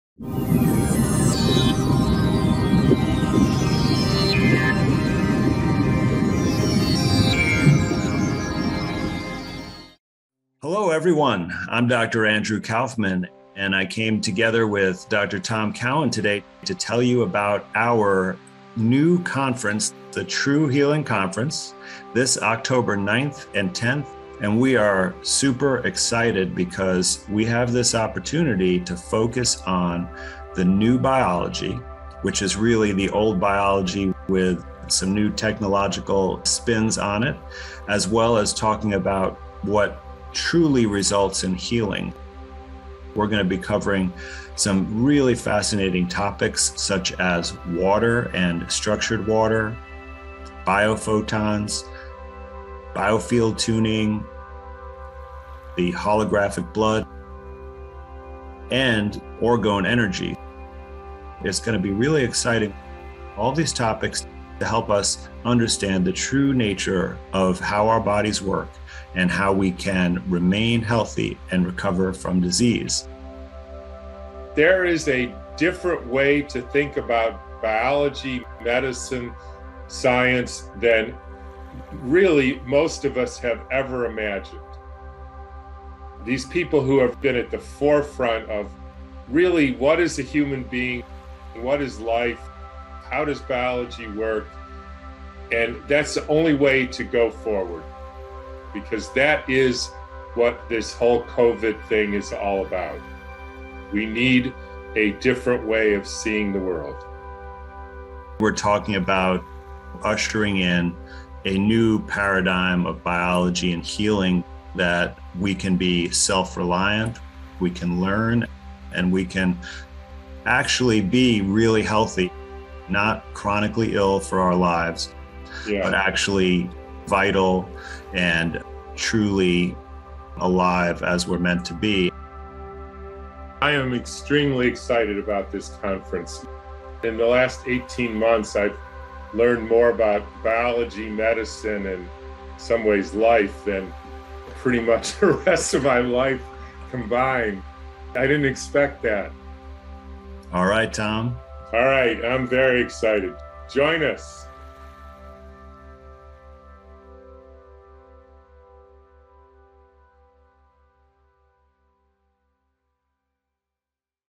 True Healing Conference 2021 | Biophotons, Orgone Energy, Cymatics, Structured Water, Healing
Whatever you think you know about health, sickness, and healing, prepare to have (some or all of) your opinions and beliefs shattered.

True Healing Conference is a gathering of some of the smartest medical minds in the world, who will share their expertise on many (truly) alternative health topics.

Join us virtually to learn about:
 ✔️the healing power of water
✔️the human biofield, cymatics
✔️orgone energy
✔️holographic blood
 ✔️bio-photons
✔️and the inherent healing forces of life.

The conference features a phenomenal speaker lineup that include:

Kelly Brogan, M.D.
Josh & Adam Bigelsen
James DeMeo, Ph.D.
Eileen McKusick
Veda Austin
Sally Fallon Morell
Andrew Kaufman, M.D.
Tom Cowan, M.D.

You will learn:

✔️Why disease is ONLY caused by malnutrition, toxins, and injury
✔️Why disease is NOT caused by bacteria, fungus, and "viruses"
✔️Body electrics such as the human biofield, cymatics, and bio-photons
✔️How to project thoughts after you have learned the language of water
✔️The truth about the "Spanish Flu", and what really happened
✔️How to construct an Orgone Energy Blanket, and what to use it for
✔️What are cymatics and how these sound waves can heal the body
✔️How to detect illness using holographic blood and live blood analysis
✔️Plus More.